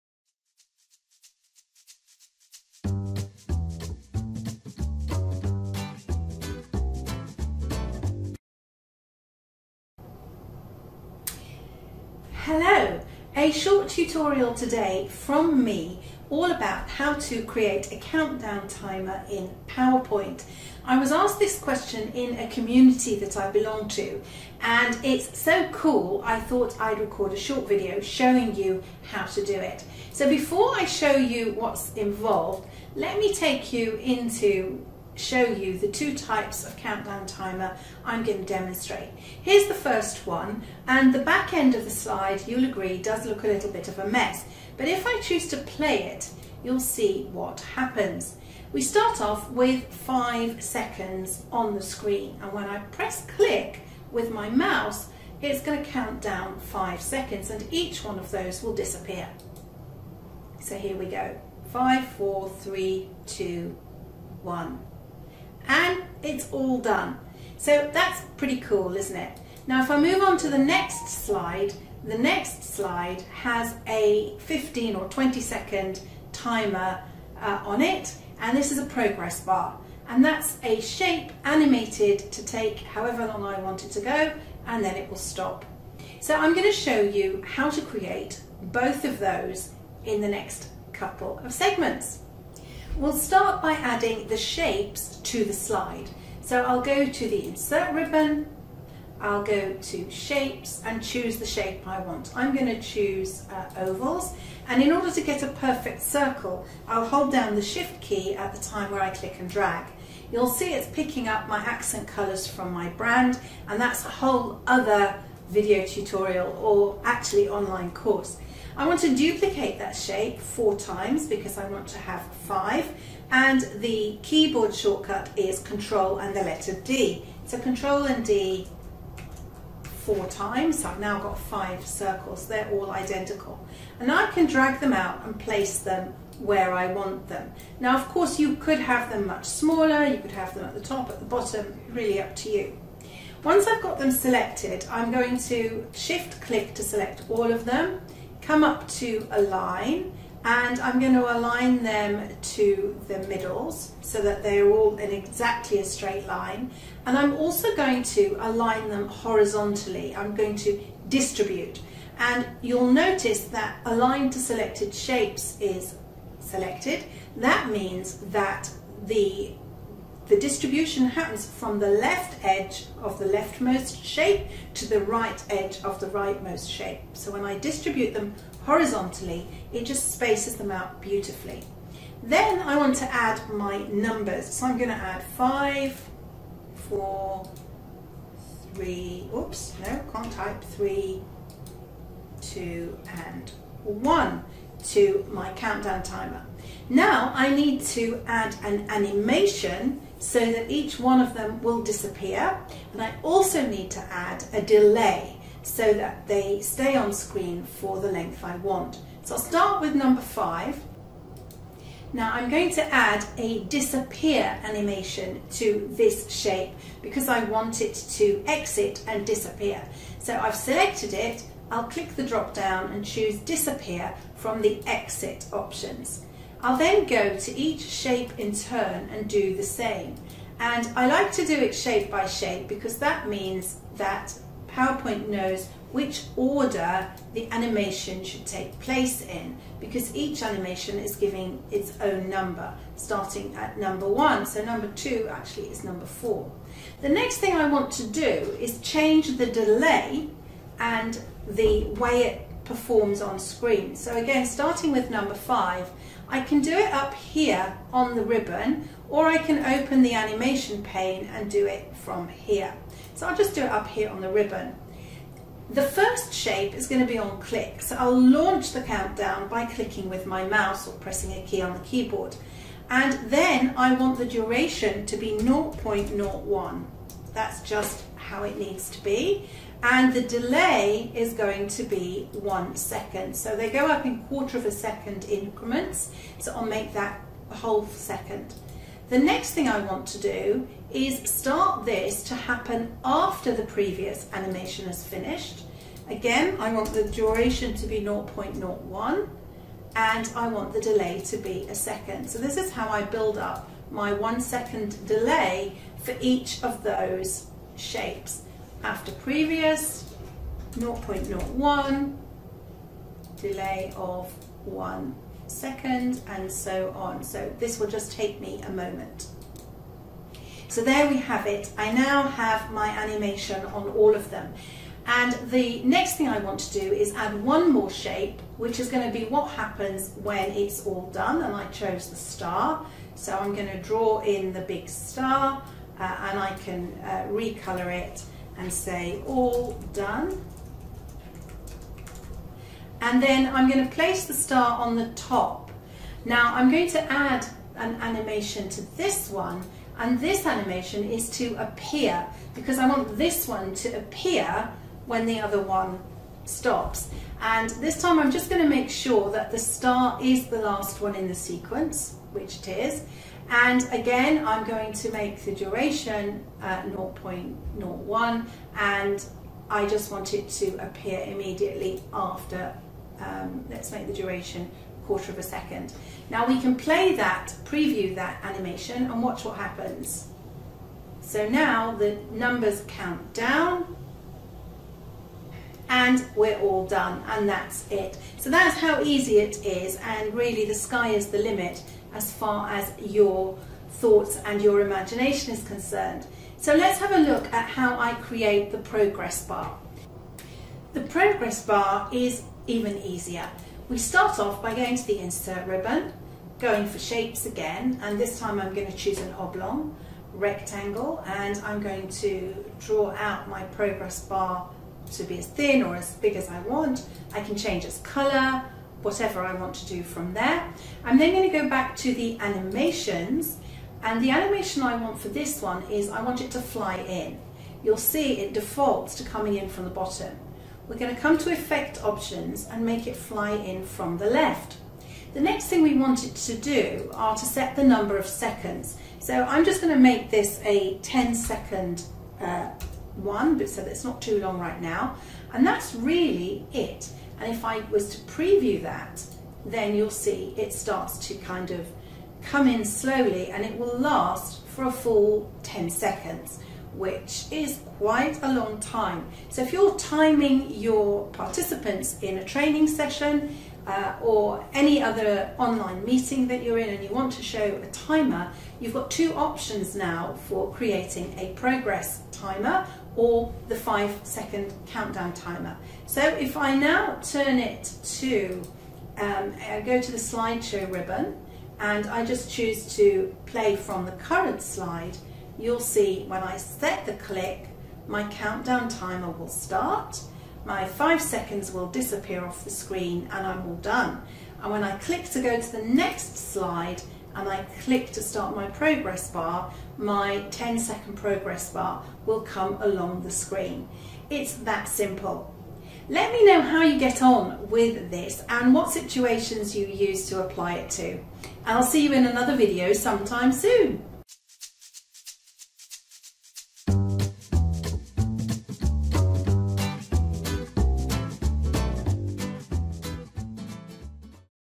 How to Create a Countdown Timer in PowerPoint
Learn how to create a countdown timer that displays on your slides in PowerPoint enabling you to set a timer for a task in a session.

In this video Shelley Fishel of tomorrow's VA shows you how to create two kinds of countdown timers, a seconds count down and a progress bar.